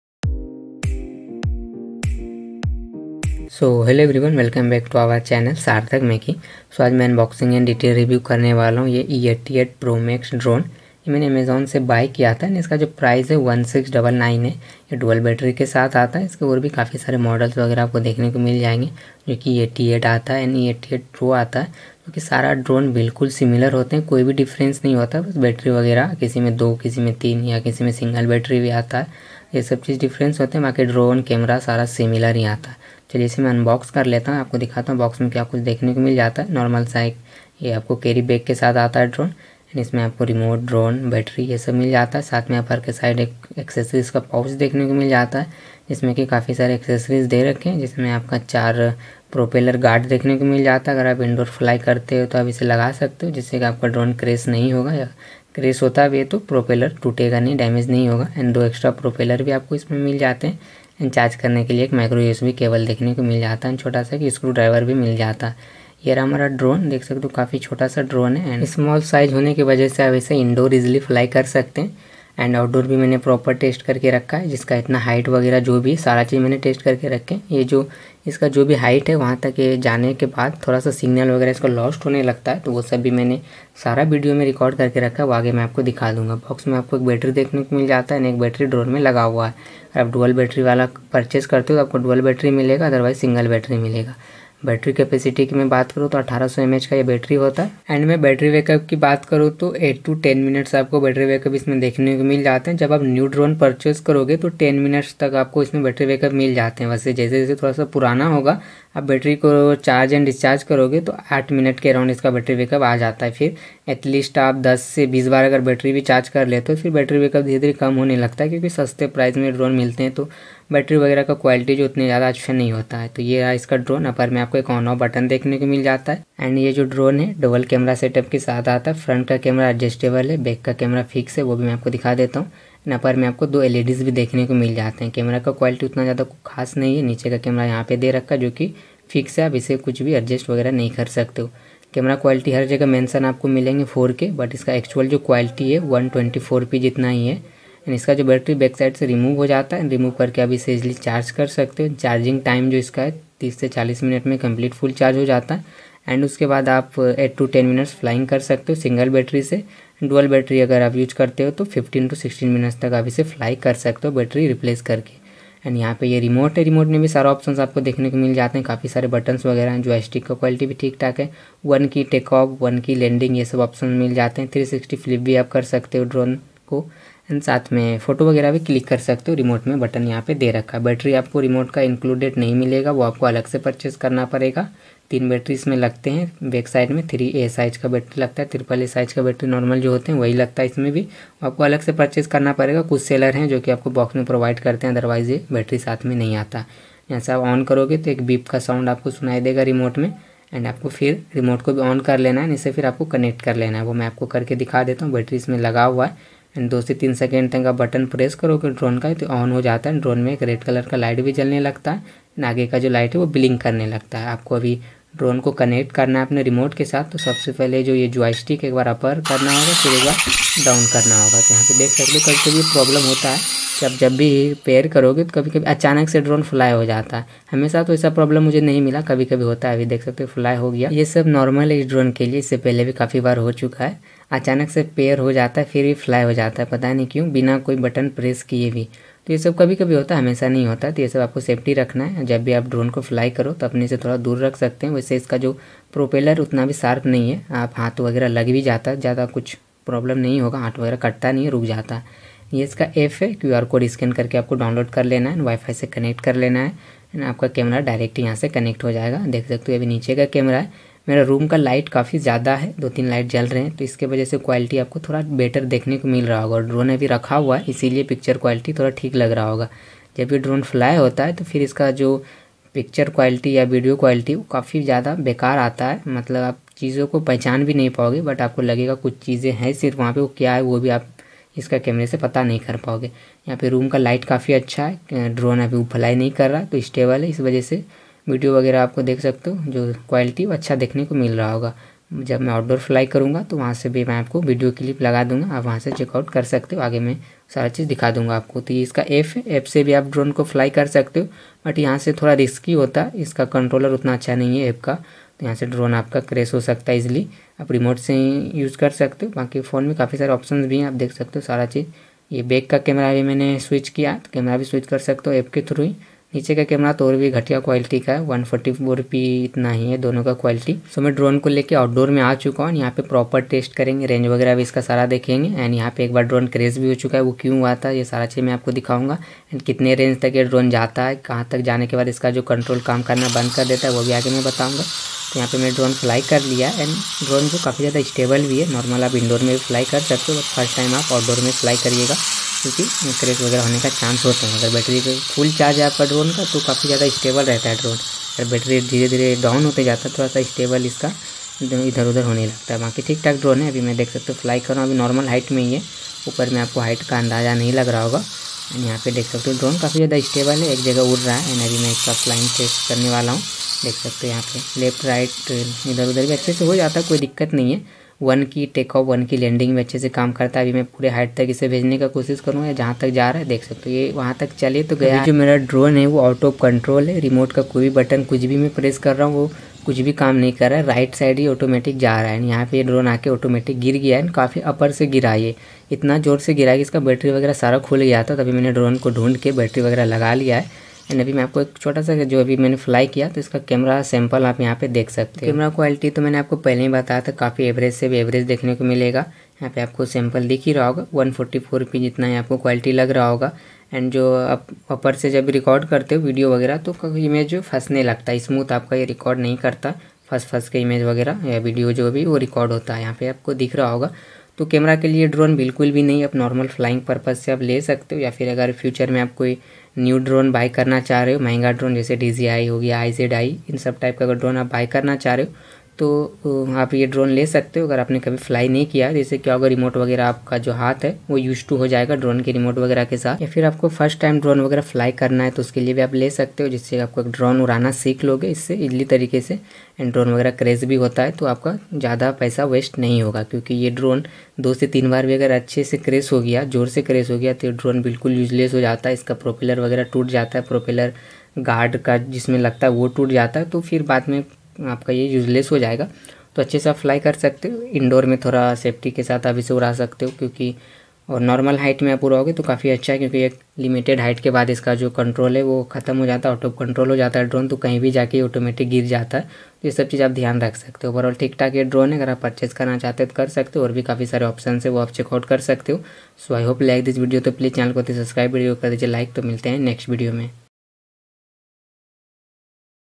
E88 PRO MAX Drone Review | Unboxing | Flying Test
Unboxing And Details Review
E88 PRO MAX Drone
With dual camera
And dual battery

Purchase link.